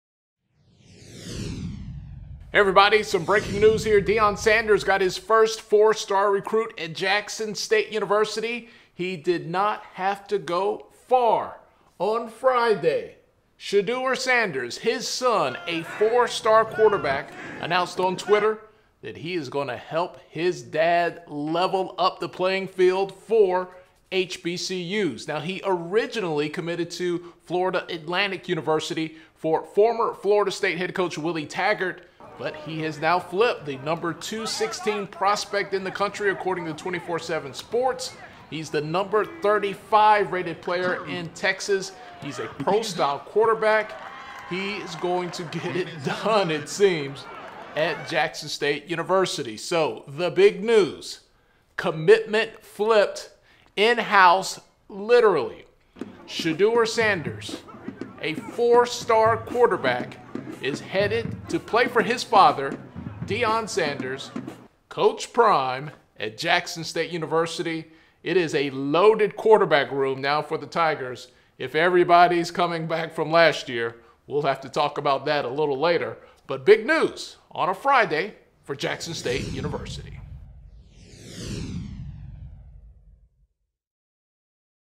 Family Affair: Shedeur Sanders joining Deion Sanders at Jackson State
Four-star quarterback Shedeur Sanders has flipped his previous commitment and is now headed to Jackson State to play for his father, Deion Sanders.